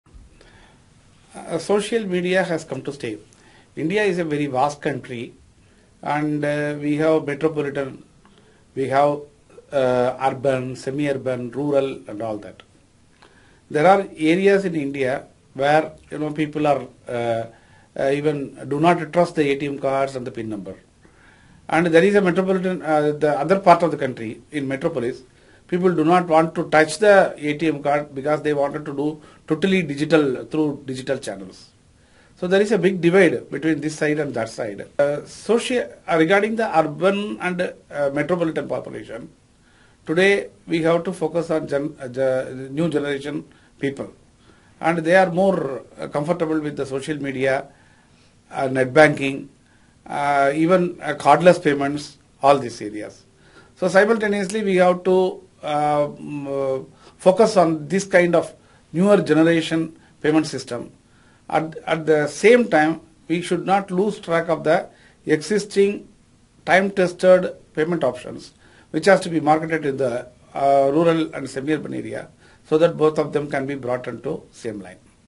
Indian Bank GM on the future of social media payments
Indian Bank GM Mr.Srinivasa Raghavan talking about the different payment needs of urban and rural areas and how social media would help address the GenY requirements.